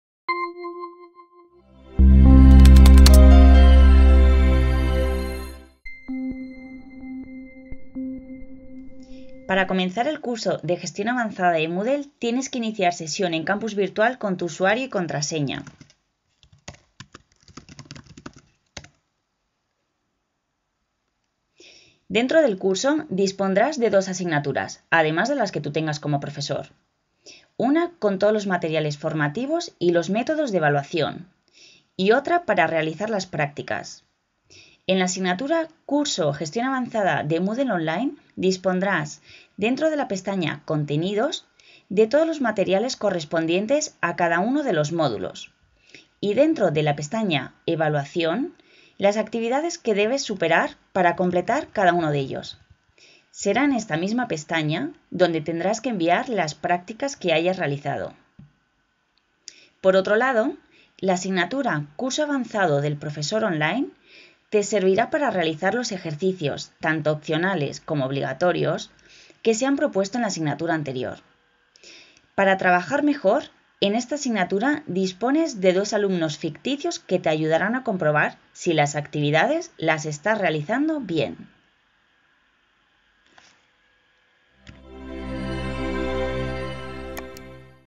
1. Información sobre las asignaturas del curso (Moodle 2.6)
Vídeo con la información de las asignaturas y los materiales que necesita un profesor para poder realizar el curso con normalidad.

Curso Avanzado de Moodle URJC